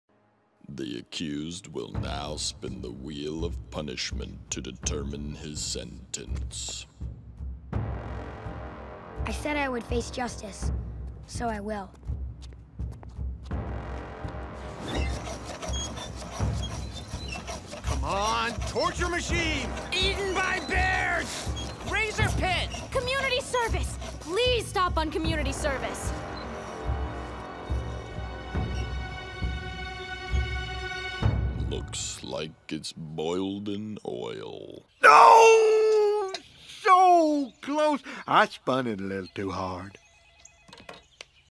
Bill spun too hard, now Aang is boiled in oil
Non-Profit of course. Just a silly little joke I made with my wife, so I decided to edit the joke here. Enjoy!